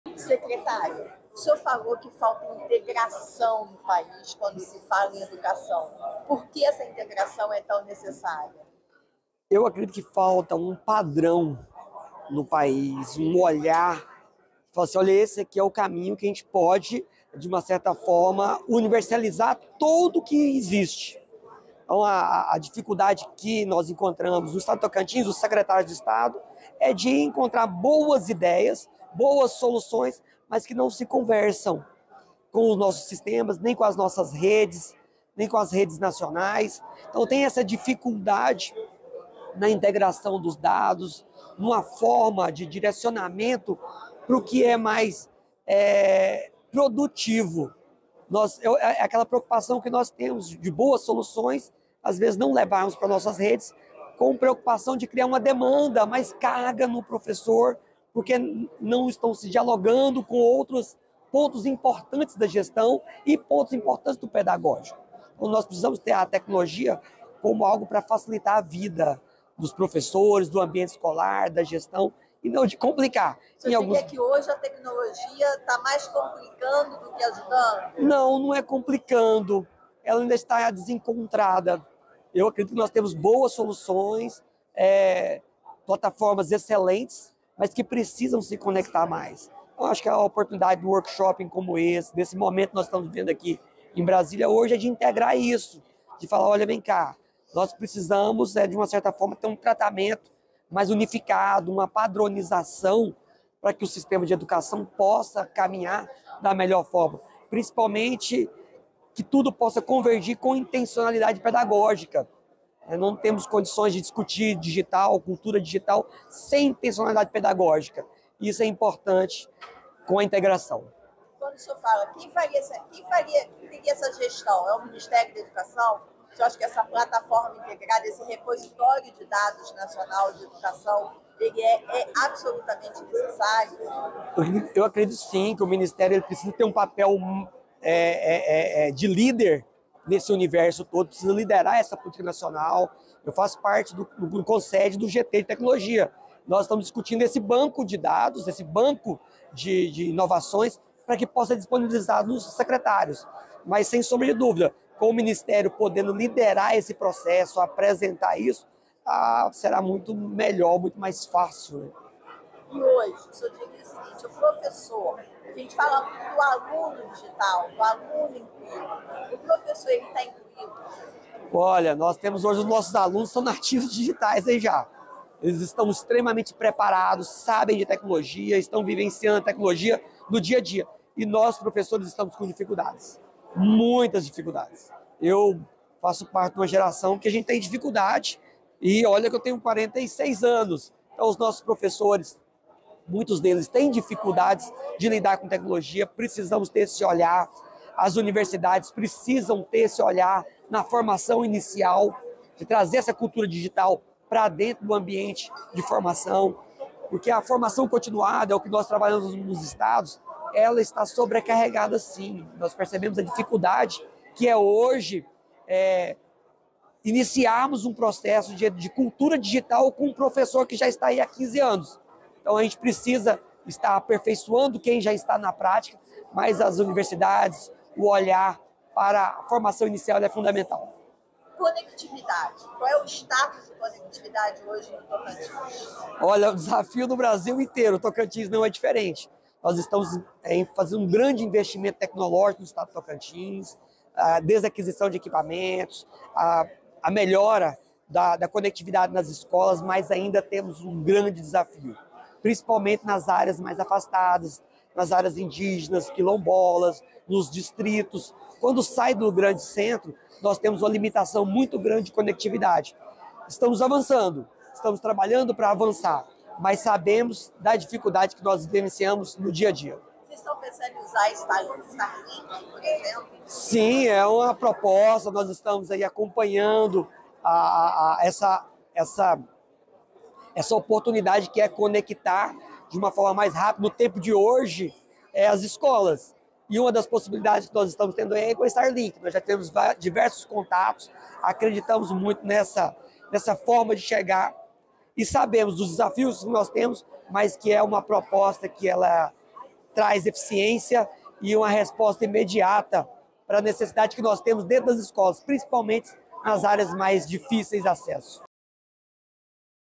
Falta um padrão para integrar os dados da Educação no Brasil
A observação é do secretário de Educação do Tocantins, Fabio Vaz. Segundo ele, há muitas plataformas no mercado, mas 'elas não conversam, não se integram e criam barreiras". Vaz participou do Workshop Educação Digital, realizado pela Network Eventos.